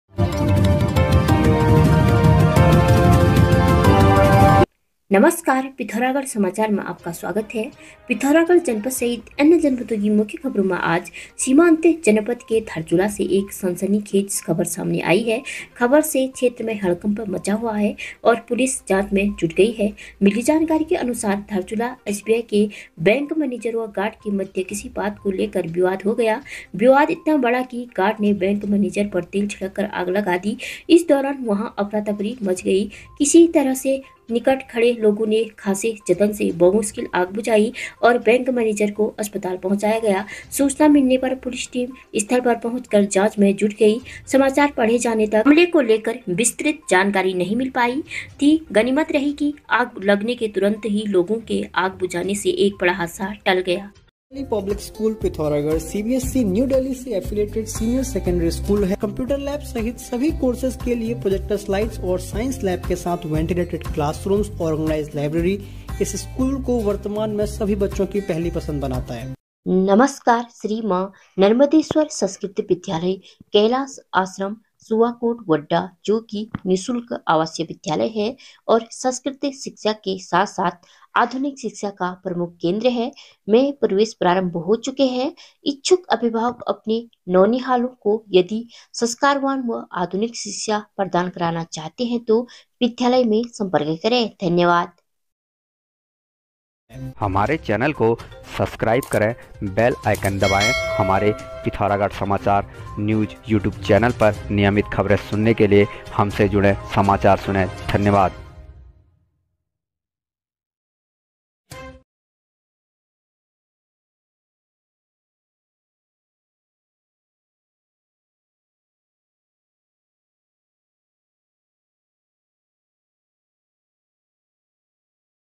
Pithoragarh : दहशत : बैंक मैनेजर व गार्ड के मध्य विवाद। तेल छिड़क कर आग
हमारे व्हाट्सएप ग्रुप से जुड़ने के लिए नीचे दिए गए लिंक पर क्लिक करें 👇